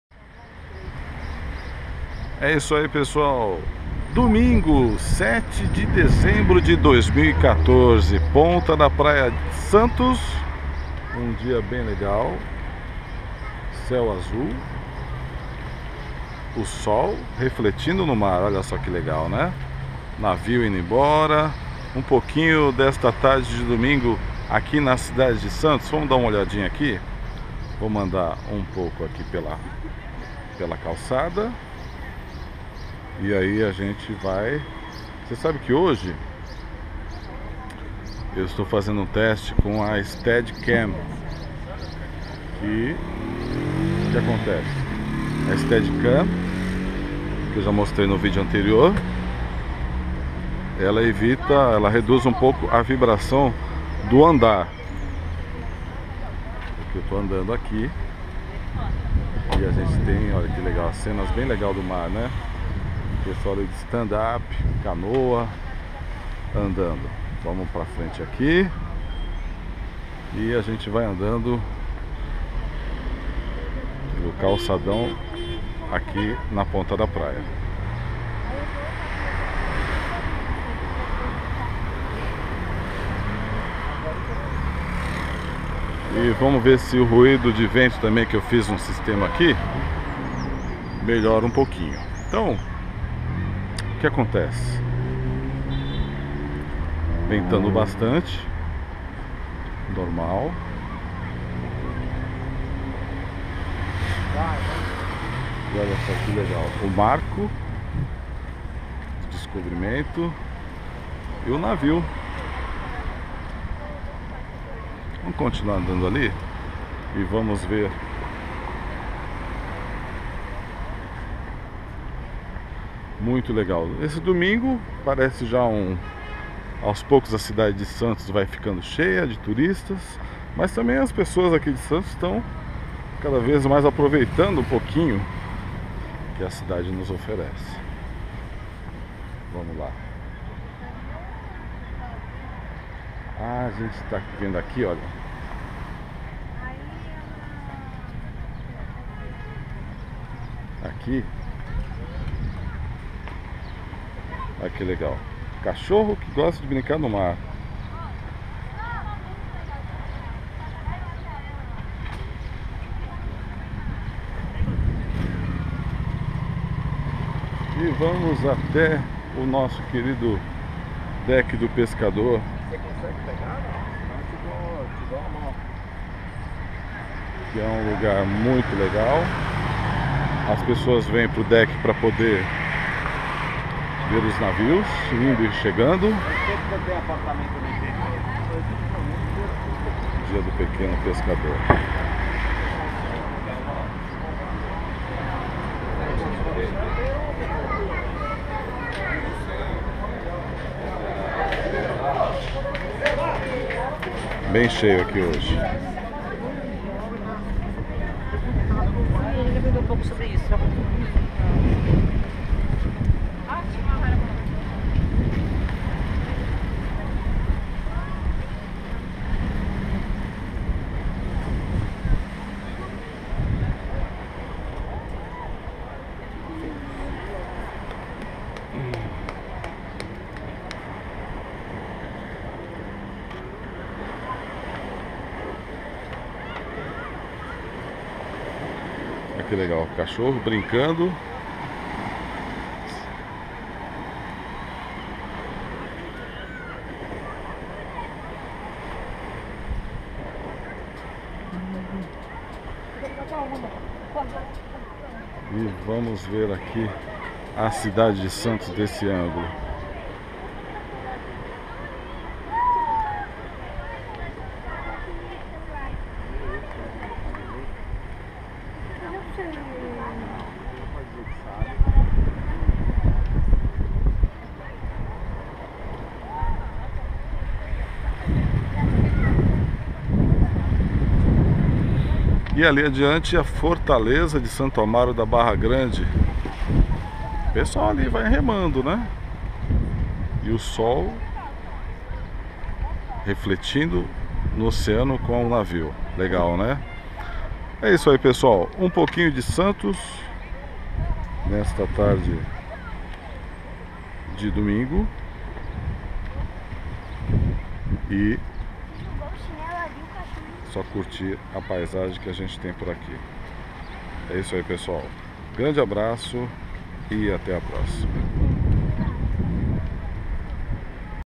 07.12.2014 Fim de tarde em Santos: testando a Steadycam
Primeiro teste outdoors com a Steadycam e a GoPro. Aproveitando o belo fim de tarde em Santos, na Ponta da Praia com o Deck do Pescador